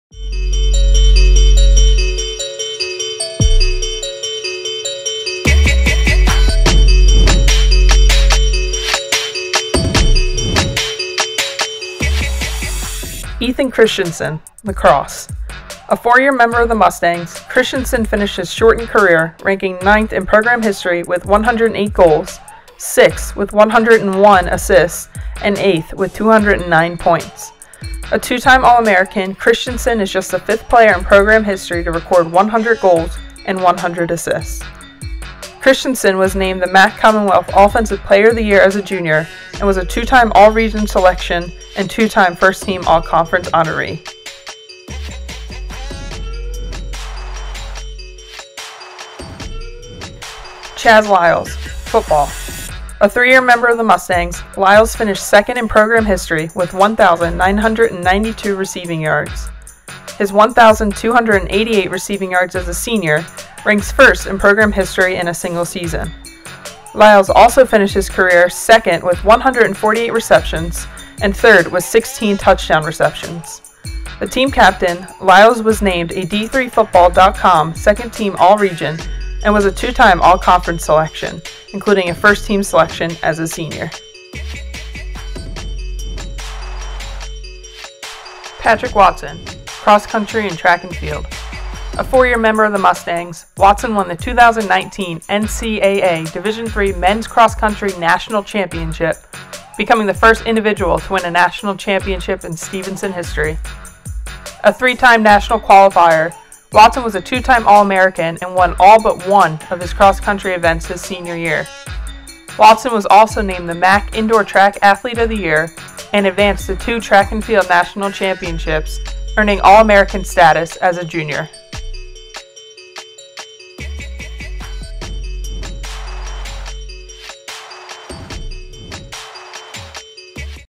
Male Senior Athlete Award Finalists
Ethan Christensen, Chaz Lyles and Patrick Watson are the finalists for the Male Senior Athlete Award